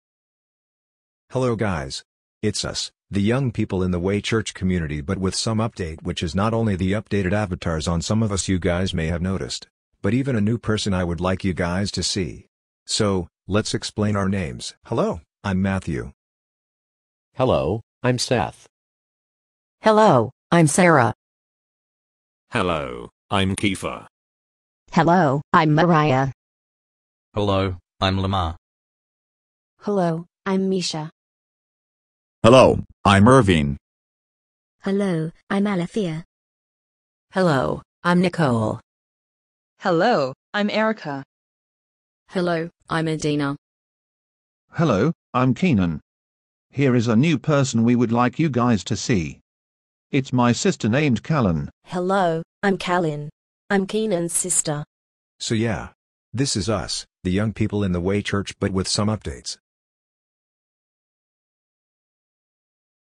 The Way Church young people (including me) in Comedy World animation but with some update
The Way Church young people are Matthew (me), Seth, Sarah, Kiefer, Mariah, Lamar, Mischa, Irvin, Alethea, Keenan, Nicole, Erica, Adina and even a new person which is Kalyn.